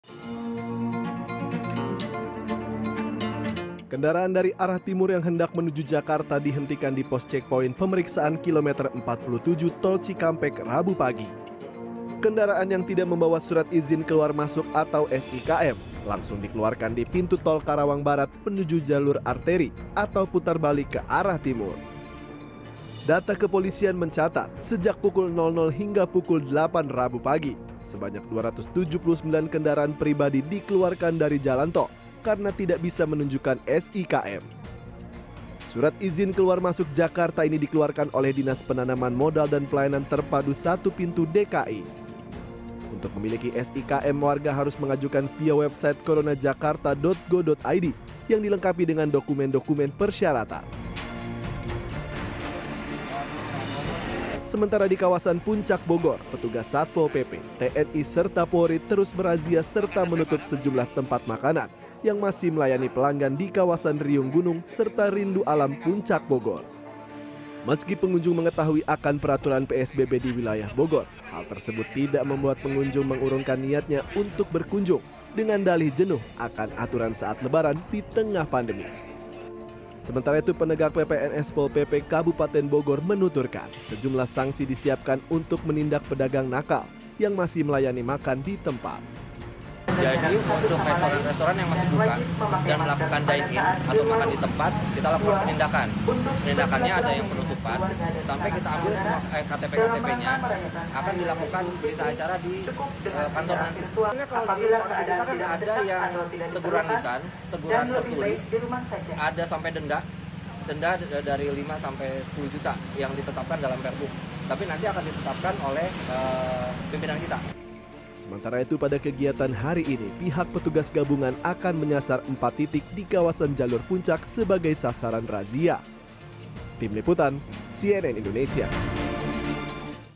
Tidak Ada SIKM, Kendaraan Dipaksa Putar Balik
Pemeriksaan surat izin keluar masuk Jakarta atau SIKM di kilometer 47 ruas tol Cikampek arah Jakarta berlangsung ketat. Pemeriksaan yang dilakukan oleh petugas gabungan juga membuat arus lalu lintas tersendat. Sementara, di kawasan Puncak Bogor, petugas Satpol PP, TNI serta Polri, terus merazia serta menutup sejumlah tempat penjualan makanan yang masih melayani pelanggan.

Like & Follow Facebook:
CNN Indonesia

cnnindonesiatv